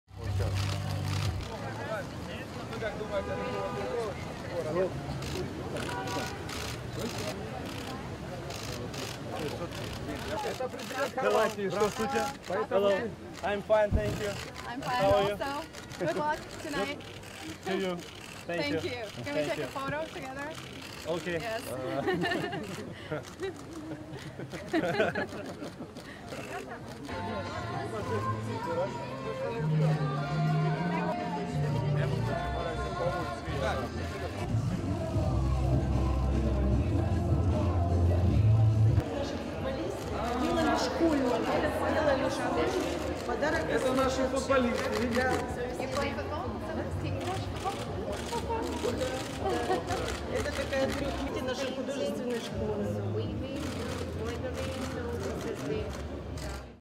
Croatian President Arrives In Sochi To Attend Croatia-Russia World Cup Quarterfinal
Croatian President Kolinda Grabar-Kitarovic arrived in the Russian Black Sea city of Sochi to support her country's soccer team, which faces Russia in the quarterfinal of the FIFA World Cup on July 7. Russian Prime Minister Dmitry Medvedev is expected to attend the match with Grabar-Kitarovic.